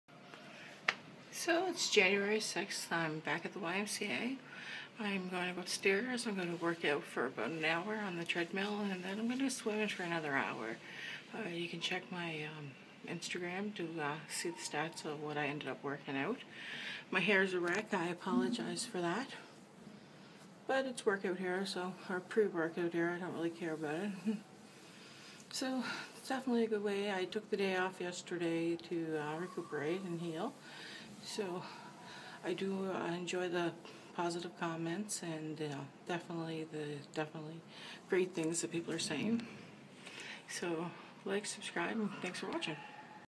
January 6, 2018 Made it to the gym another day
I am trying to loss some weight and live a more healthy life , would love too hear from others on same path